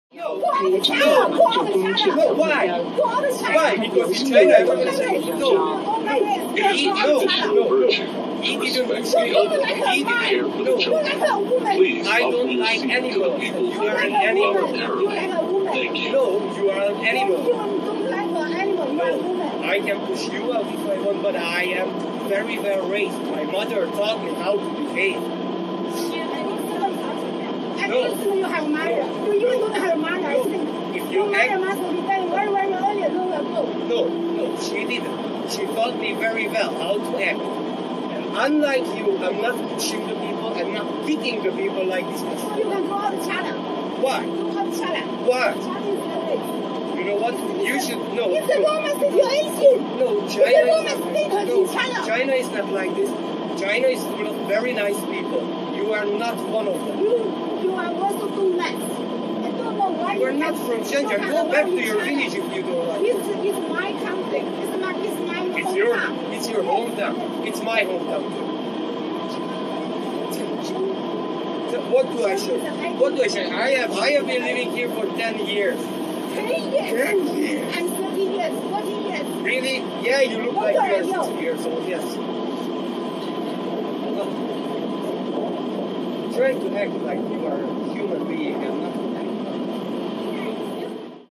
深圳女子地铁大骂老外滚出中国！网友听后表示：这老外挺讲理的
1月13日，广东深圳。一名女子和男性外国人在地铁上发生了争吵，女子怒火冲冲地叫男子滚出中国，而男子则不停询问她为什么，女子则诋毁男子不是个男人，倒像个女人。对此男子反驳称女子跟动物一样，没有一点教养。